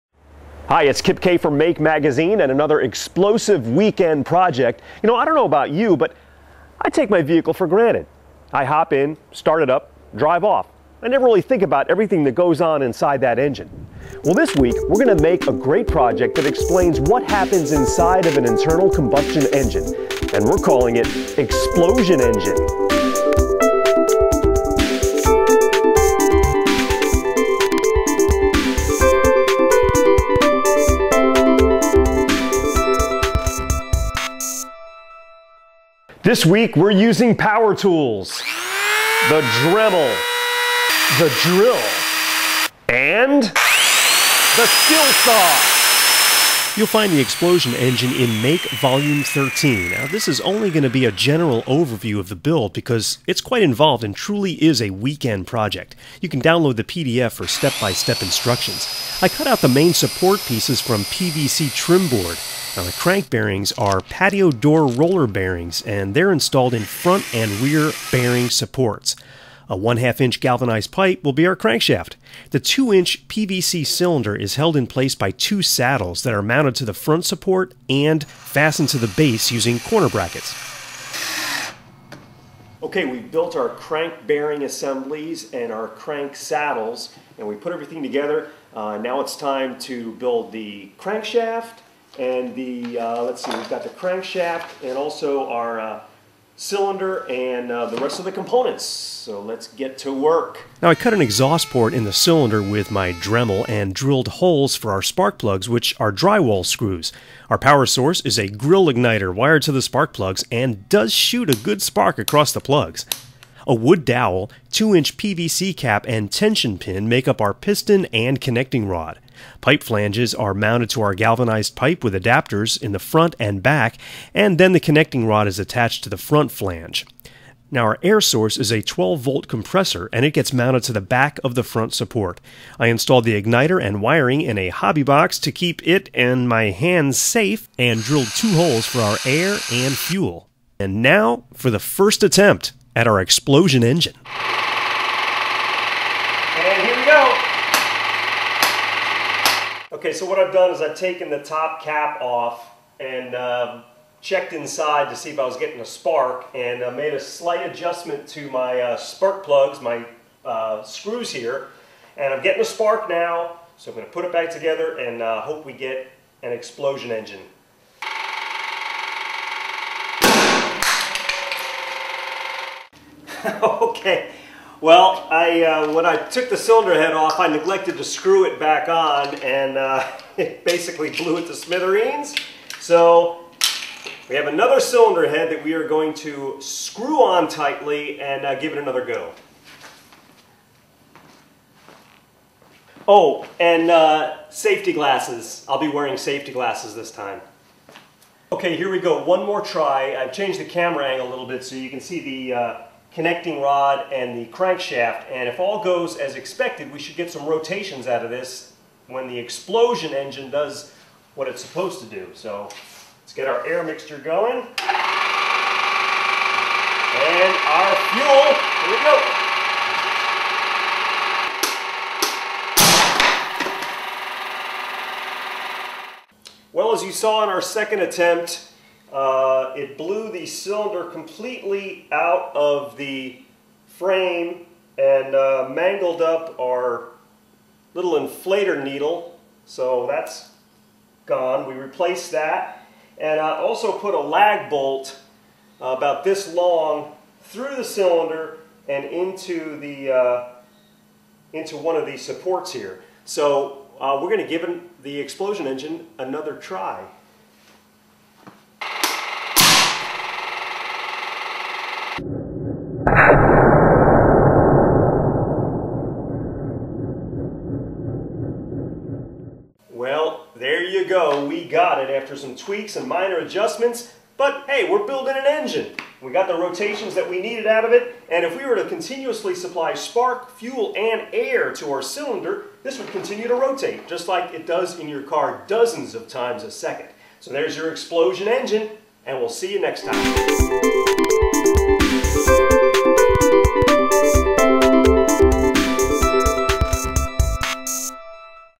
Weekend Project: Make an Explosion Engine
Make a DIY internal combustion engine that explodes into action just like the real thing! See the misfires and final test results!